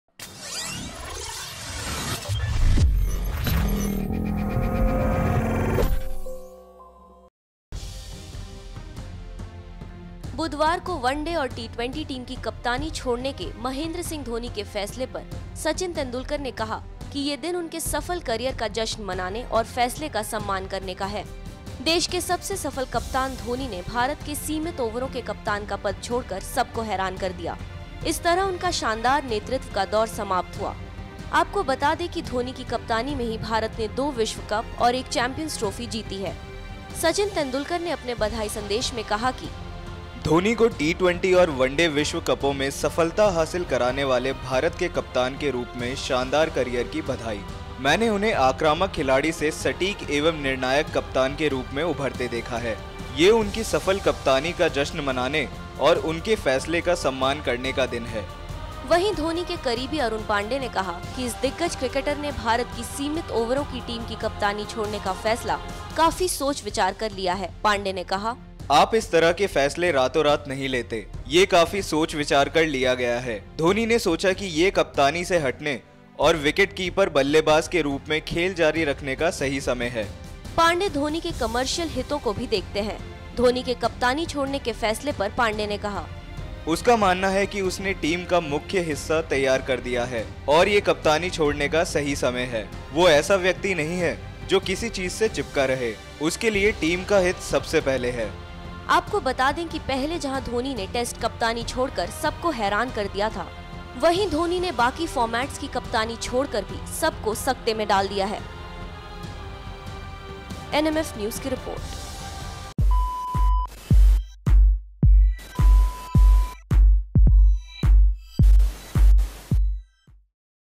M S Dhoni के Support में आए Sachin Tendulkar, कहा- Dhoni के फैसले का सम्मान करें
The sporting fraternity to congratulated Mahendra Singh Dhoni on his glorious captaincy career with cricket legend Sachin Tendulkar leading from the front, saying "it's a day to celebrate his successful career and respect the decision. Dhoni, the country's most successful skipper, today sprang a major surprise by stepping down as India's limited overs captain, thereby bringing an abrupt end to his glorious leadership tenure.